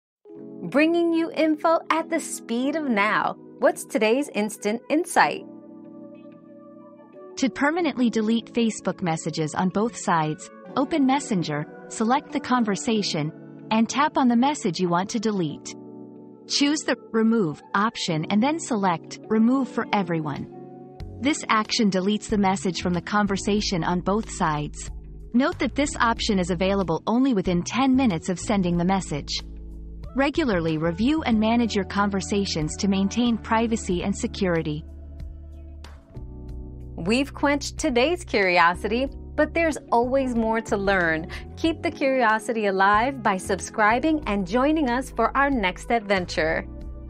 How to permanently delete Facebook messages on both sides?
Mastering Facebook Messaging: How to Permanently Delete Messages on Both Sides 👉 Delete Messages Easily 👉 Learn the step-by-step process to permanently delete Facebook messages on both sides within seconds. Safeguard your privacy and maintain security by managing your conversations effectively.

Laura S. Harris (2024, June 3.) How to permanently delete Facebook messages on both sides?
    🌐 AskAbout.video